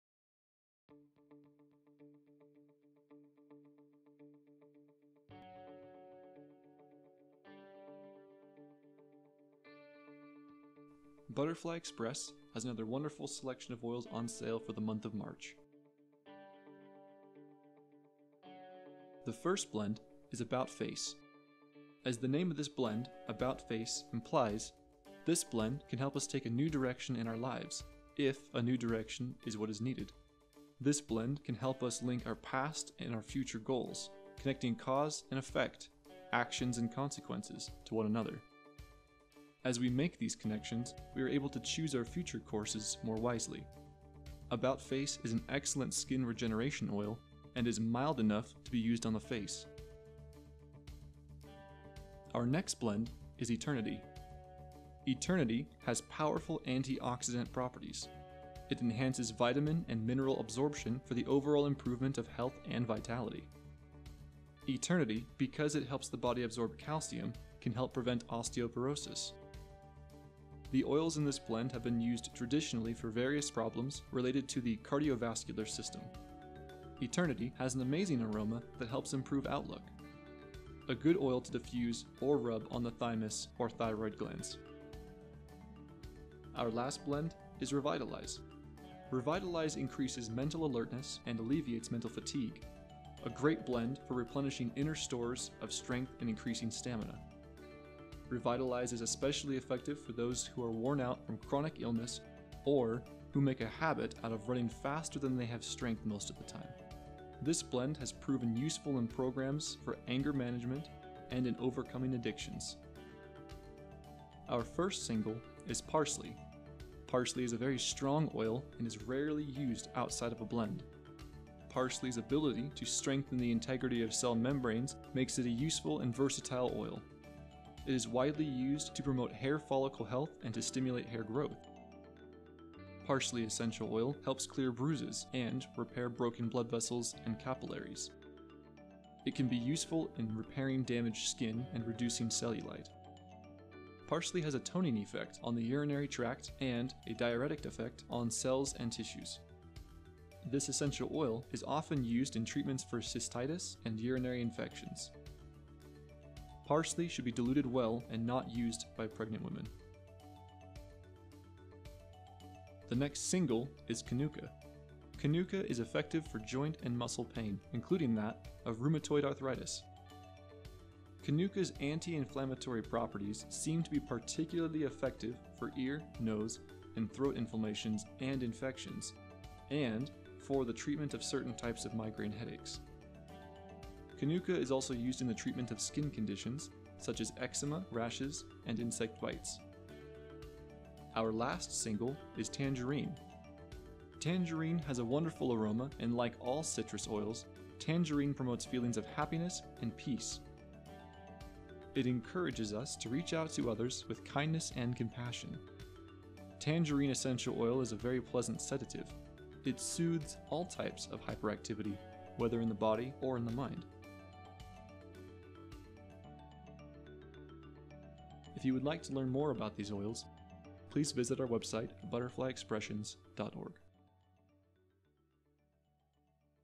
Butterfly Expressions March Specials 2018
March is here, and another selection of essential oils have been put on sale for you. Each has been carefully chosen, and each is equally wonderful!